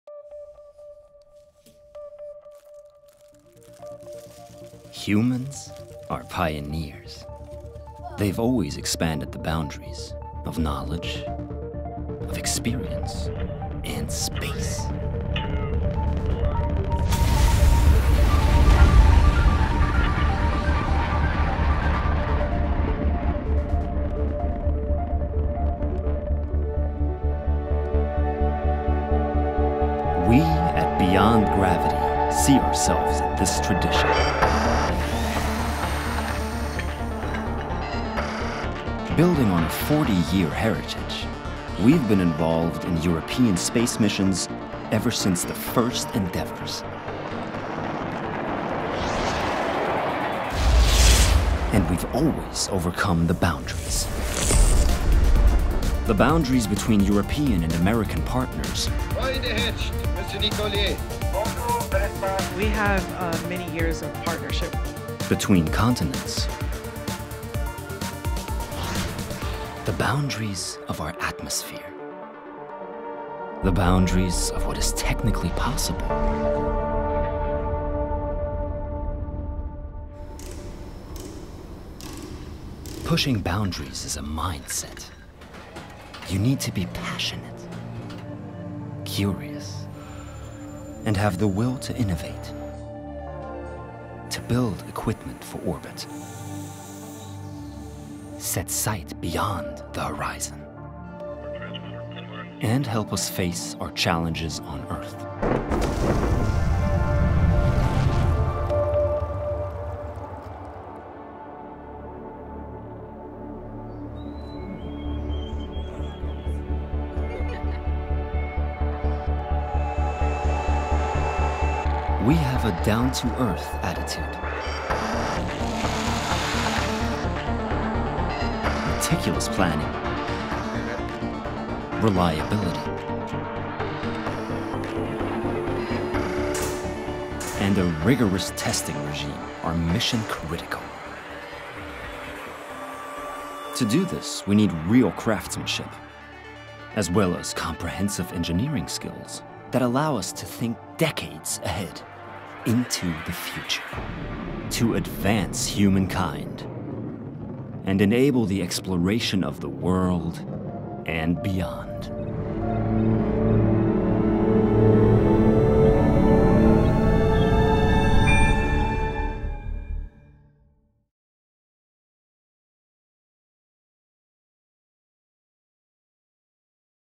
Our Story.
Humans are pioneers. They have always expanded the boundaries of knowledge, experience and space. We see ourselves in this tradition. We have been involved in space missions ever since the first endeavors – and we have always overcome the boundaries. The boundaries of our innovative home country, the boundaries between European and American partners, the boundaries of our atmosphere, the boundaries of what is technically possible. Pushing boundaries is a mindset. Anyone who flies customers into orbit and sets their sights beyond the horizon needs to be passionate, curious and have the will to innovate in order to meet the challenges that new worlds present. At thesame time, we are not daredevils, but have a down-toearth attitude.

A contradiction? No: because reliability, meticulous planning and a rigorous testing regime are mission critical. Necessary to allow us to turn any mission to overcome new frontiers into a calculable risk for our customers. To do this, we need craftsmanship as well as comprehensive engineering skills that allow us to think decades ahead. To realize what’s next beyond the limits of what’s currently feasible. Together with partners in Europe and the U.S., customers on every continent, and as part of the inhabitants of “Spaceship Earth,” we push boundaries of what’s imaginable – to reach new horizons, to advance humankind, and to enable the exploration of the world and beyond.

Are you onboard?